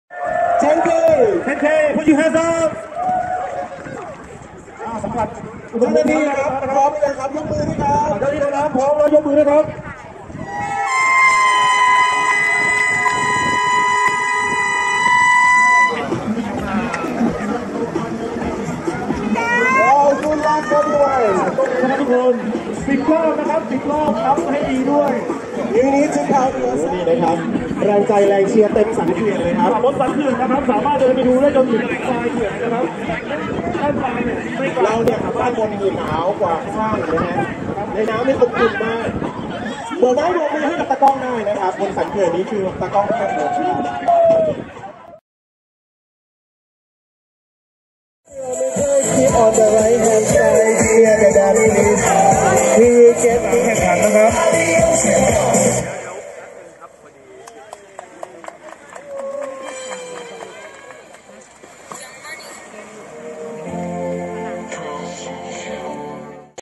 泰國芭達雅海泳馬拉松10公里組出發(Thailand Swimathon 2019)
悠泳部落客12月7-8日組隊參加：『泰國芭達雅海泳馬拉松(Thailand Swimathon)』~